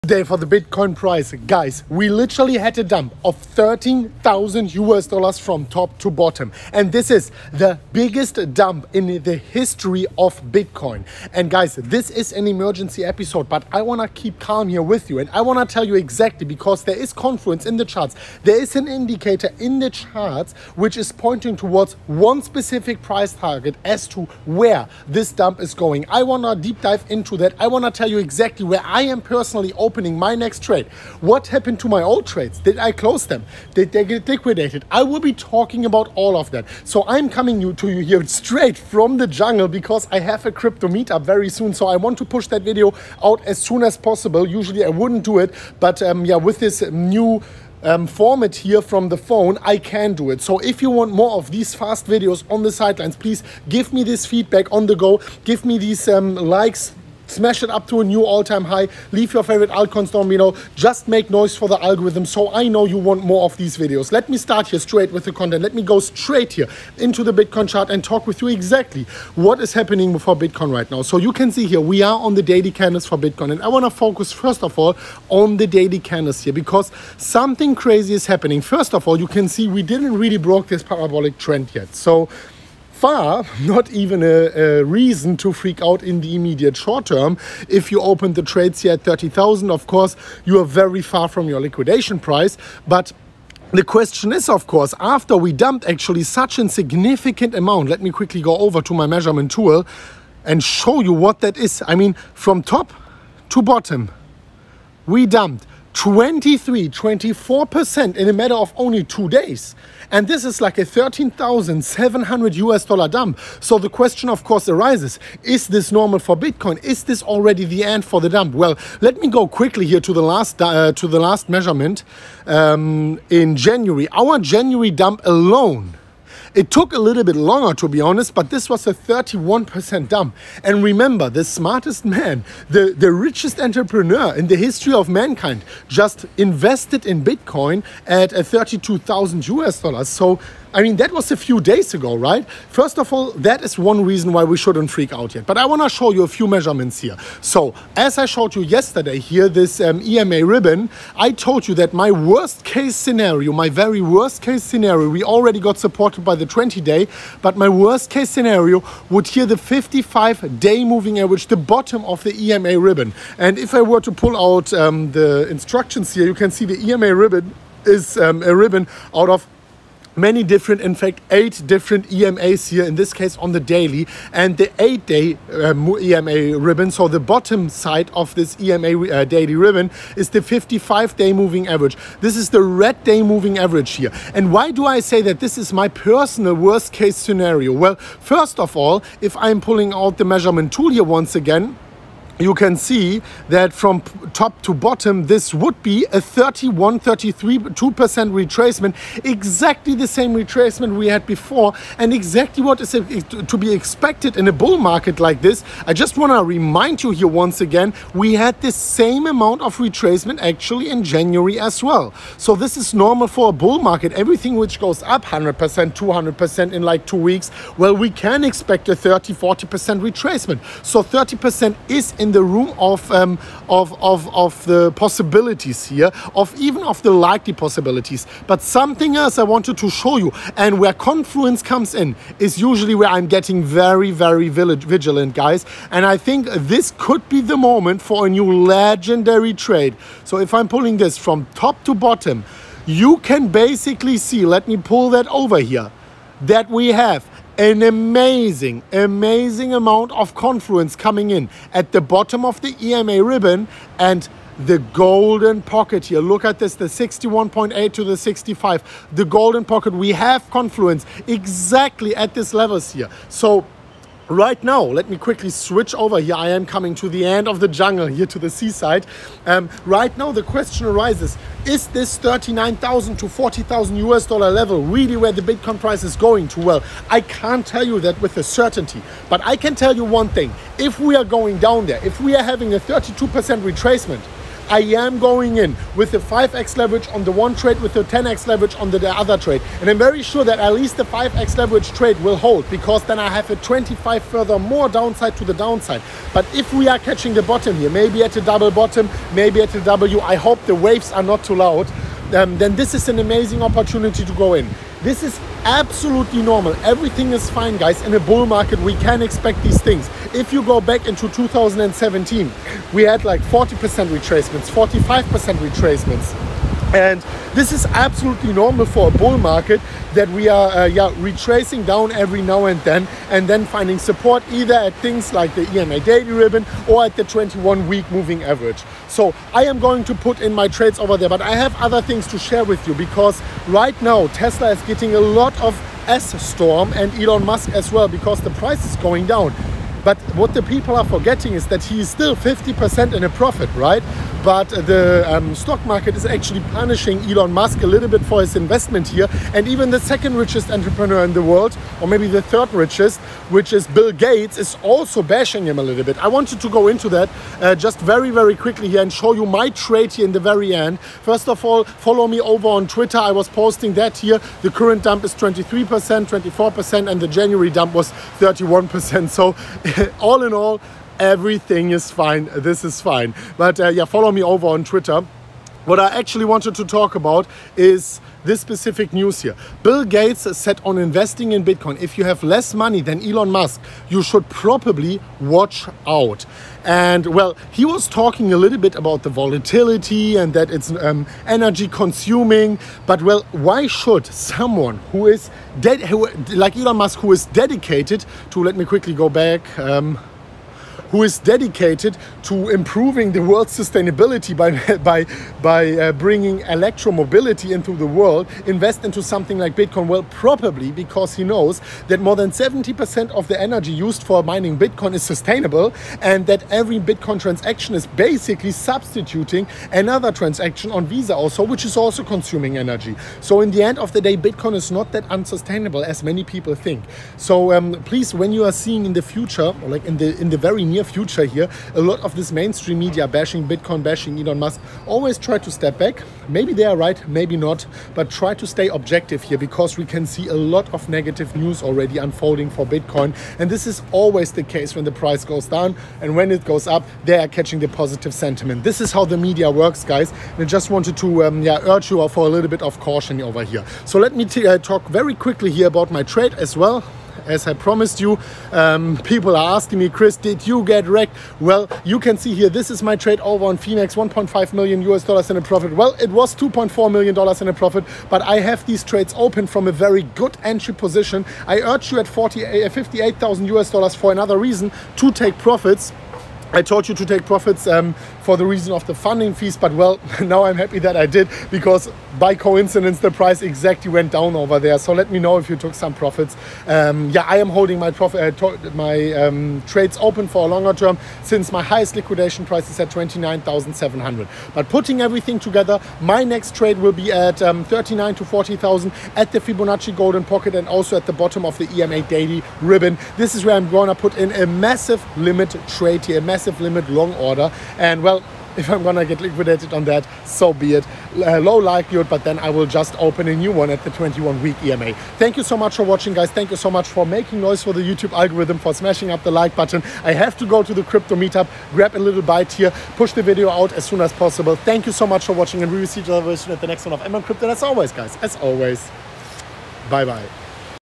⚠️I AM BUYING THE BIGGEST BITCOIN DUMP IN HISTORY HERE!!!!!!!!!!!! [exact price..]
$1'180 BONUSES BELOW!
[after initial deposit]

(PLEASE: _*ONLY*_ for EXPERIENCED TRADERS!!)

*the PrimeXBT Deposit Bonus can be used to cover fees

Timestamps:
00:00 Intro
01:05 HOW LOW WILL BITCOIN GO??
04:27 THIS IS NORMAL
06:20 BILL GATES BASHING ELON MUSK!!!
07:20 MY TRADES!!! (Did I get rekt?)

MMCrypto Bitcoin Crypto (YouTube Channel)